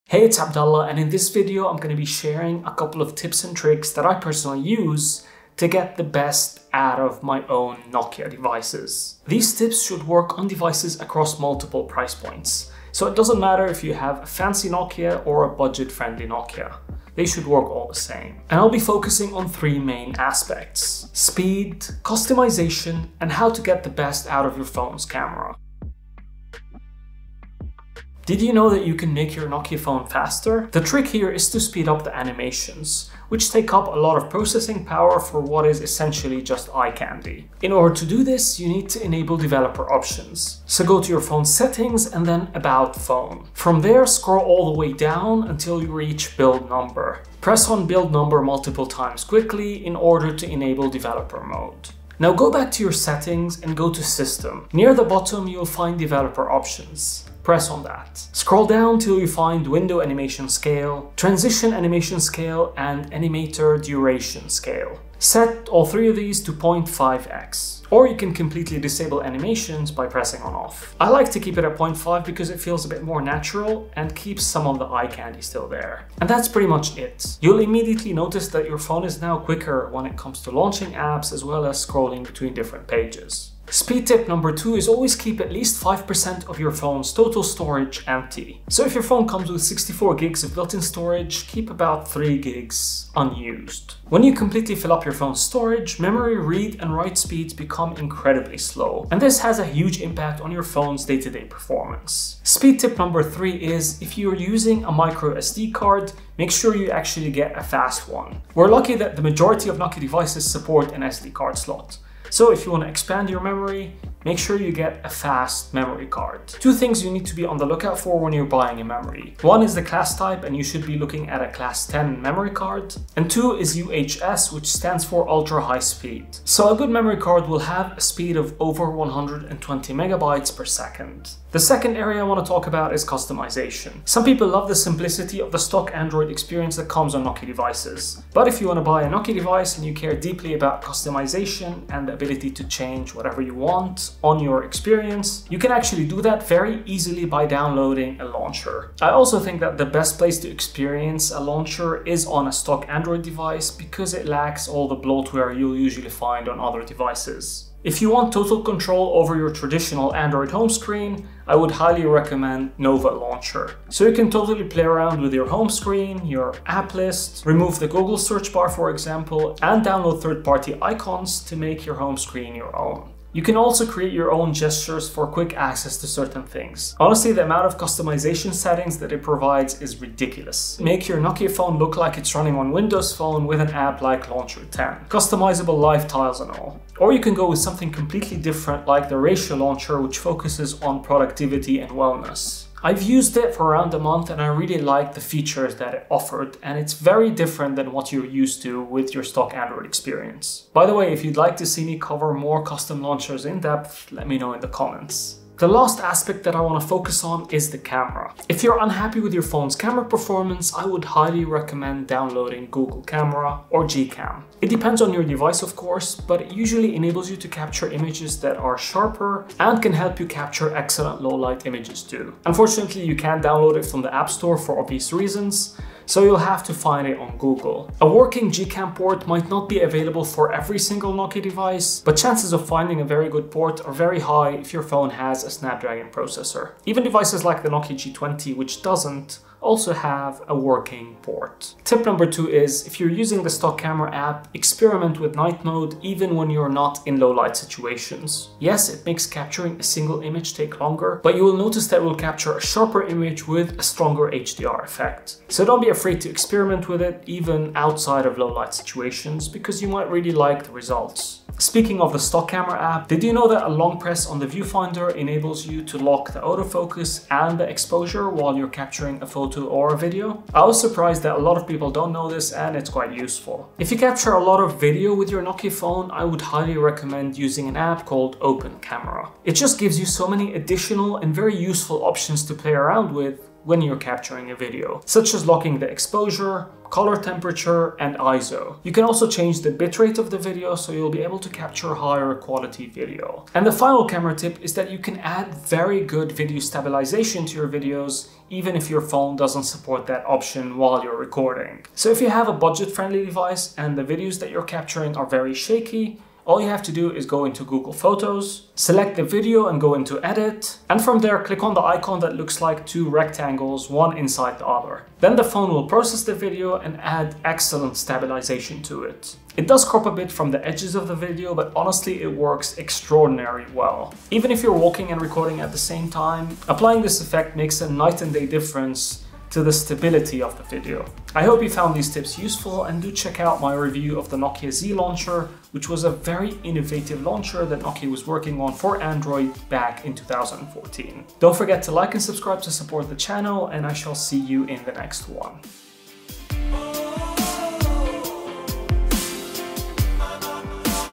Nokia Tips & Tricks | Improve Speed, Customization, and Camera!
This is a tips and tricks video for Nokia Android phone users. I share with you what I do to improve my Nokia phone experience, by making my Nokia phones faster, with more customization options (my favorite launchers) and some camera tips and tricks to get the most out of your Nokia phone's camera. You can improve your shaky footage by adding stabilization, improve the quality of the videos you capture and more.

00:00 - Introduction
00:25 - Improve Speed
01:23 - Storage Tip
01:42 - SD Card Tip
02:11 - Improve Customization
03:25 - Improve Camera
04:53 - Add Stabilization